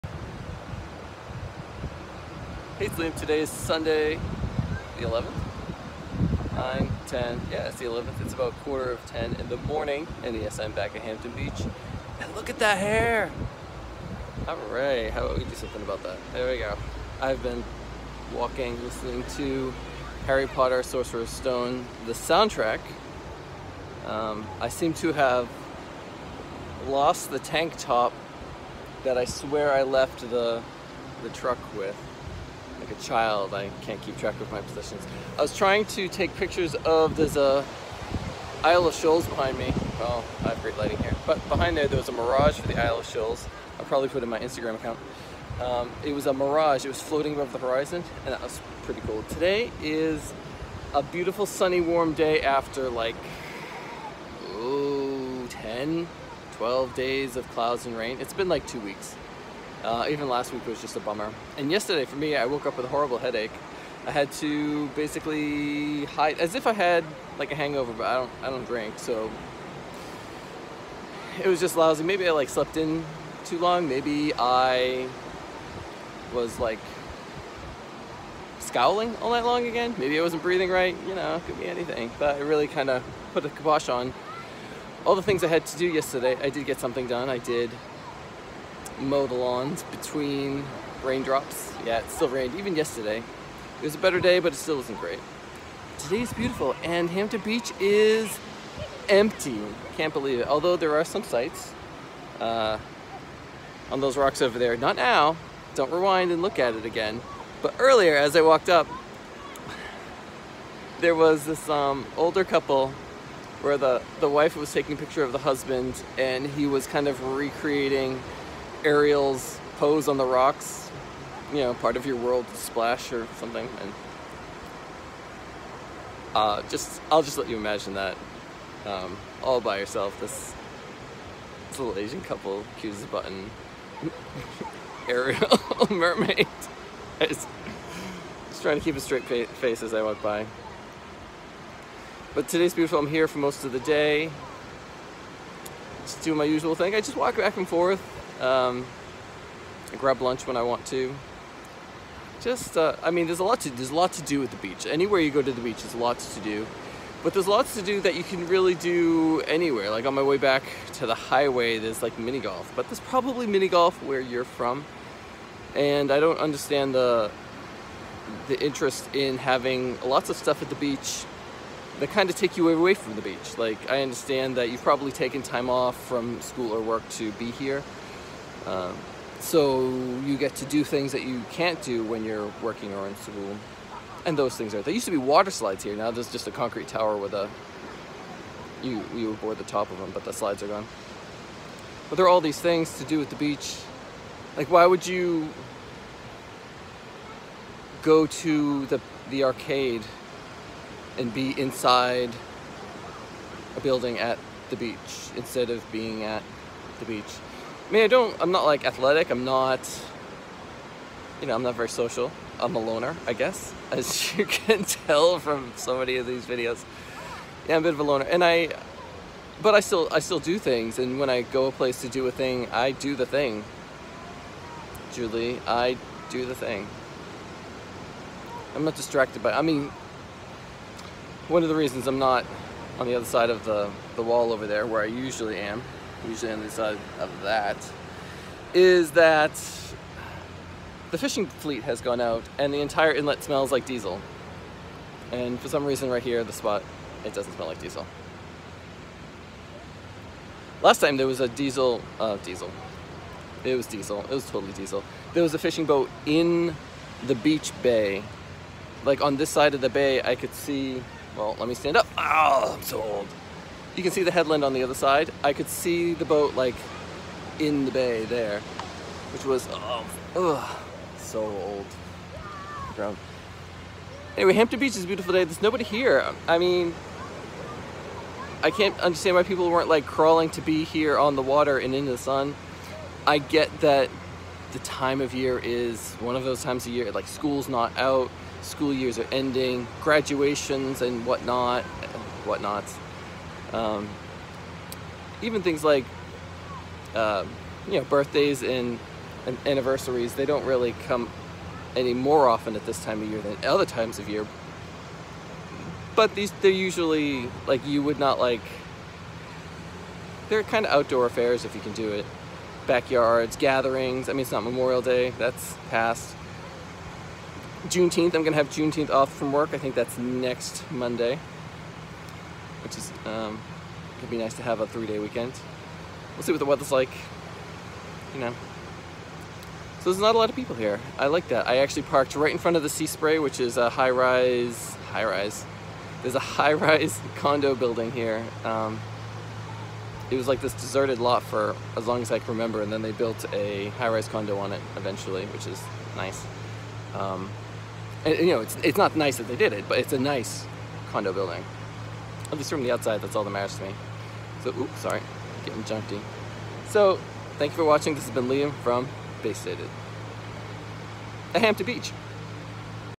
Bay Liam - Do The Thing (you know, the thing that I do at the beach)
I dropped my tanktop (sleeveless shirt) only 40 paces onto the sand but it was still there when I retraced my steps back to my truck. Checkout my IG baystated to see some pics of the cold water mirage of the Isles of Shoals. I couldn’t get them to appear well on video. There were also mirages tithe south of Cape Ann. A lovely day at Hampton all in all.